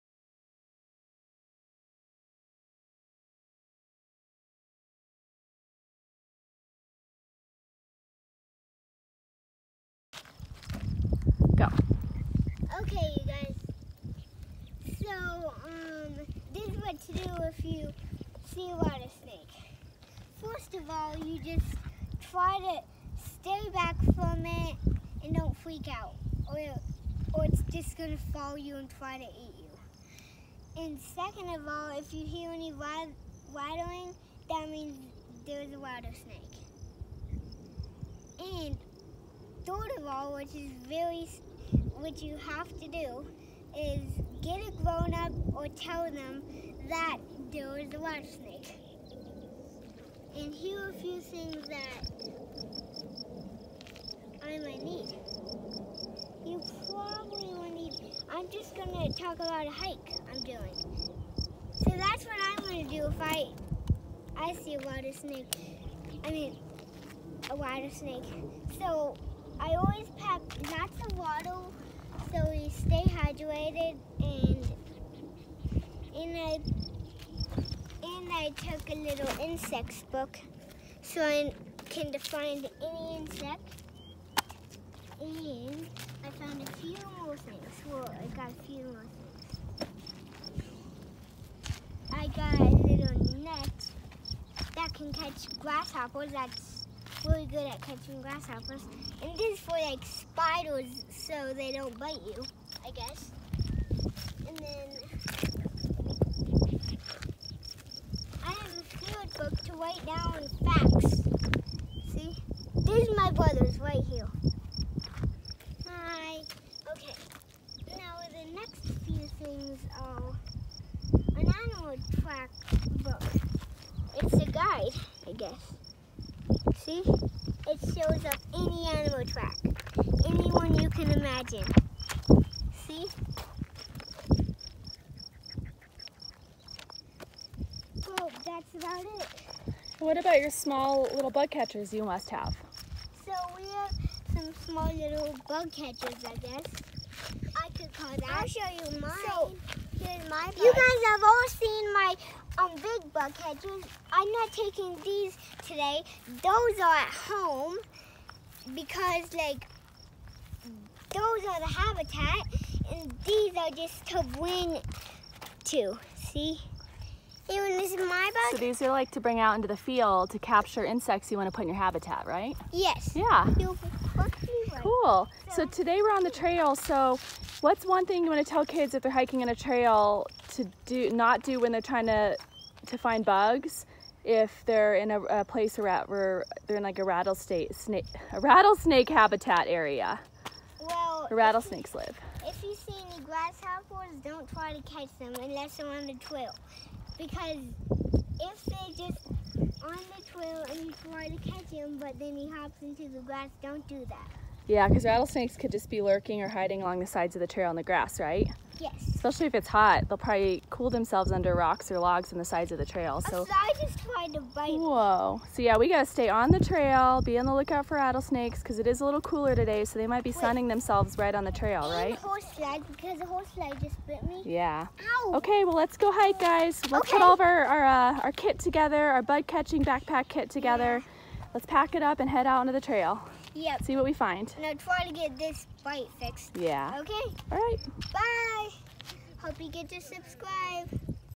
What to pack on your bug catching adventure + rattlesnakes!
Wyatt talks about what he brings on hikes and how to avoid rattlesnakes when searching for bugs on the trail in Colorado.